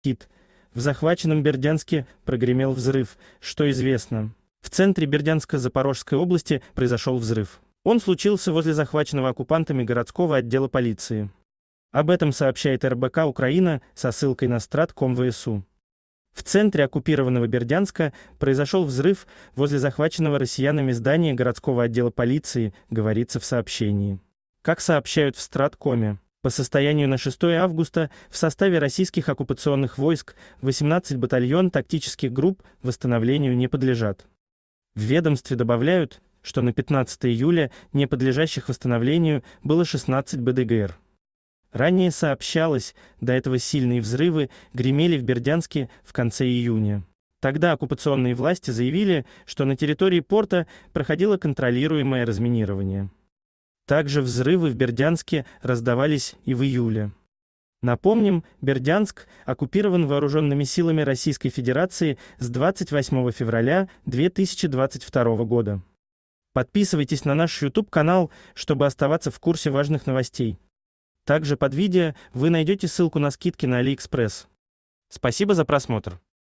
В захваченном Бердянске прогремел взрыв: что известно.
Бердянск Запорожскаяобласть Получи скидку на Алиэкспресс по этой ссылке до 70% + дополнительные скидочные купоны от продавцов. В центре Бердянска Запорожской области произошел взрыв. Он случился возле захваченного оккупантами городского отдела полиции. Об этом сообщает РБК-Украина со ссылкой на СтратКом ВСУ. "В центре оккупированного Бердянска произошел взрыв возле захваченного россиянами здания городского отдела полиции", - говорится в сообщении. Как сообщают в "СтратКоме", по состоянию на 6 августа в составе российских оккупационных войск 18 батальон-тактических групп восстановлению не подлежат. В ведомстве добавляют, что на 15 июля не подлежащих восстановлению было 16 БТГр. Ранее сообщалось, до этого сильные взрывы гремели в Бердянске в конце июня. Тогда оккупационные власти заявили, что на территории порта проходило